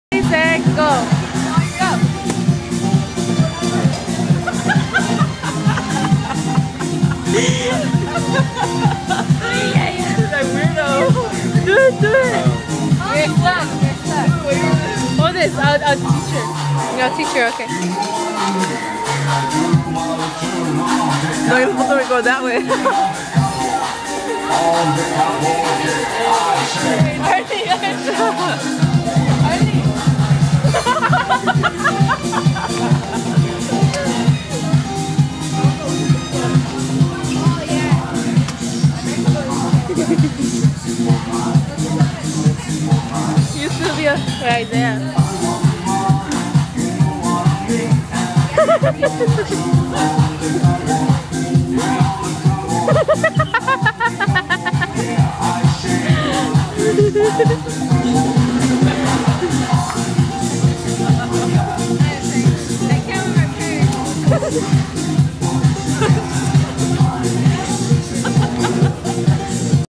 YOLO @ Citywalk
Robot dance with friends (:
Ft. Silvia . Diana. & Jenni